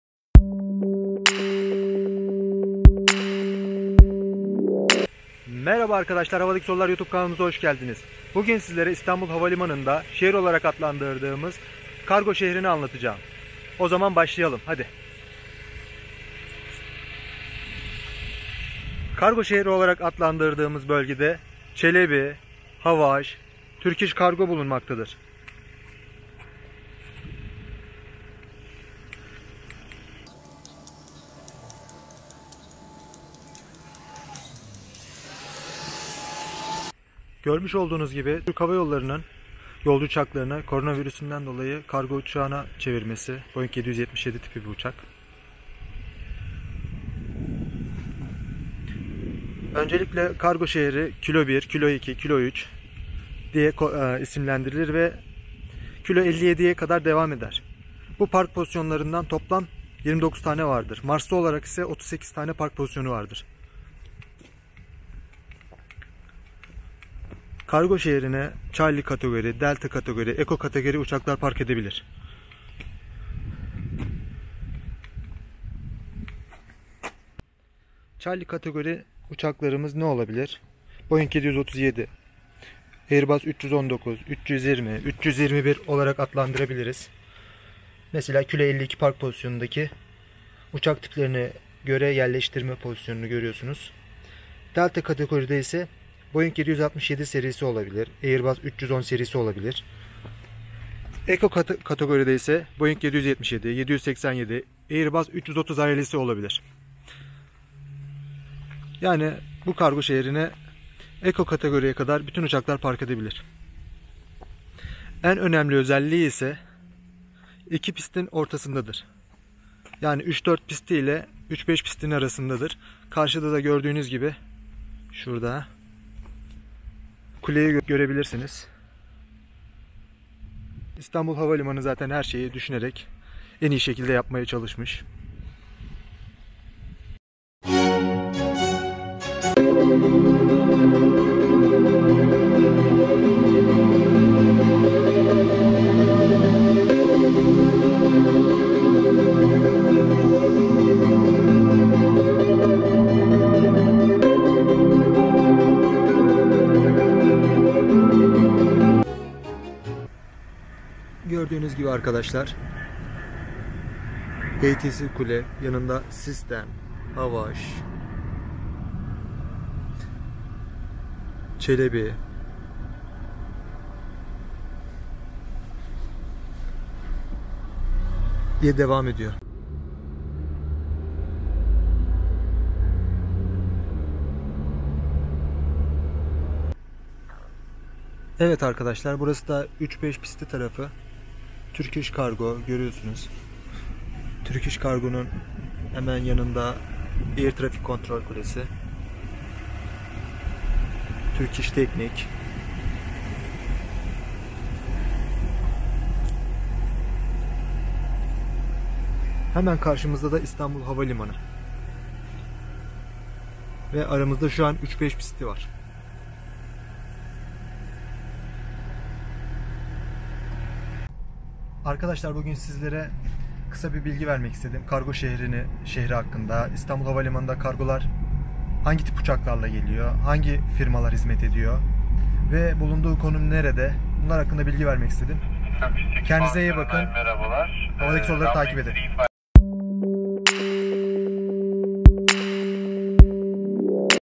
İSTANBUL HAVALİMANI KARGO ŞEHRİ
İstanbul Havalimanı'nda Kargo Şehrini Sizlere Tanıtmak İstedim
2 pist arasında bulunan(34 pist ve  35 pist) kargo şehri
Birçok firma tarafından operasyon sürdürülmektedir.
Uçak kategorileri